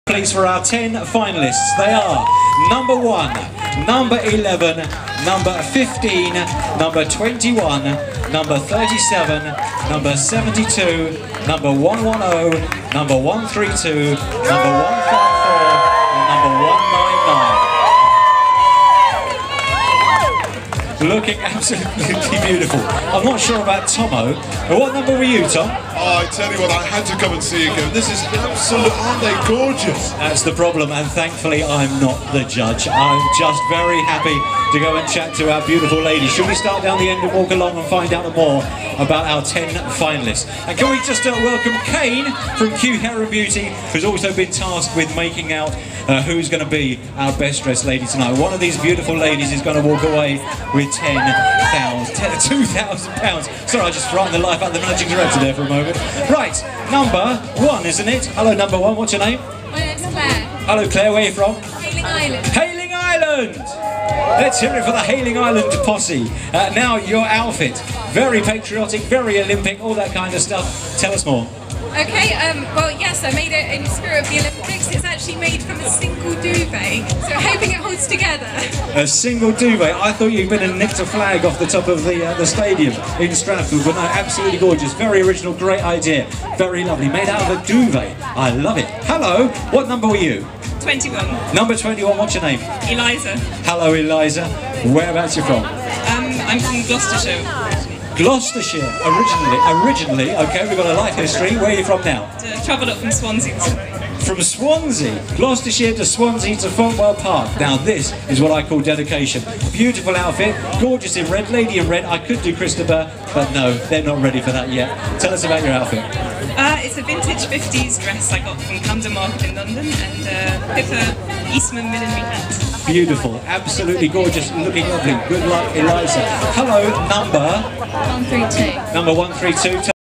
The ten finalists in the Best Dressed lady Fontwell 2012
Read and see more in videos and photos here on Eliza's blog

With £2000 up for grabs to the winner the top ten are selected from hundreds of entries. Eliza is in the final. The judges have made a great selection from lots of different styles on show. They now have an extremely difficult task to narrow down the winner. but all ten win a bottle of champagne... Q Hair and Beauty of Chichester are sponsoring the award. Here we see two of the contestants interviewed ..Eliza and a lady who has made an incredible union jack dress herself...from a duvet!